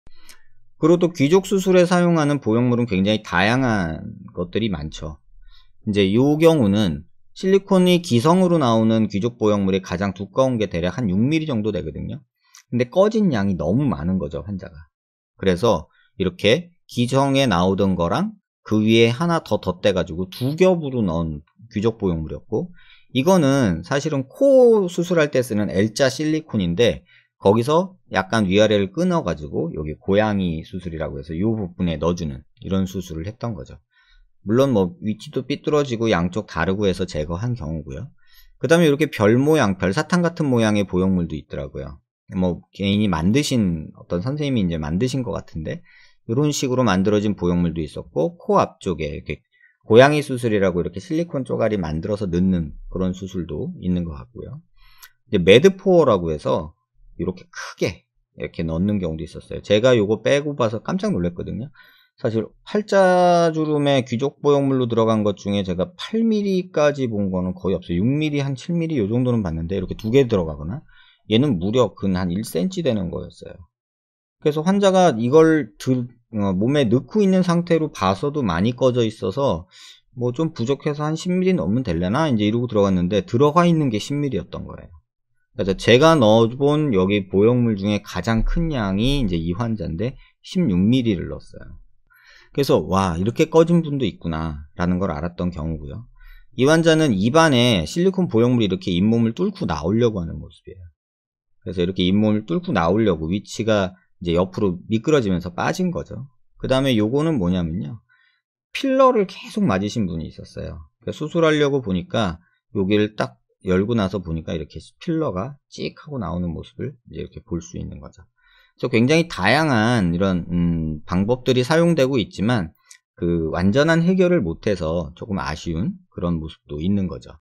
(ENG SUB) 귀족수술에 사용되는 다양한 보형물과 실재 예 [팔자주름의 원인 및 해결방법 #12]
[에이치성형외과 백정환 성형외과전문의 동영상설명]

유튜브 라이브 귀족수술/노블캣수술 설명

귀족수술에 사용되는 다양한 보형물과 실재 예

5f, 509, Gangnam-daero, Seocho-gu,
Seoul, Korea (Banpo-dong)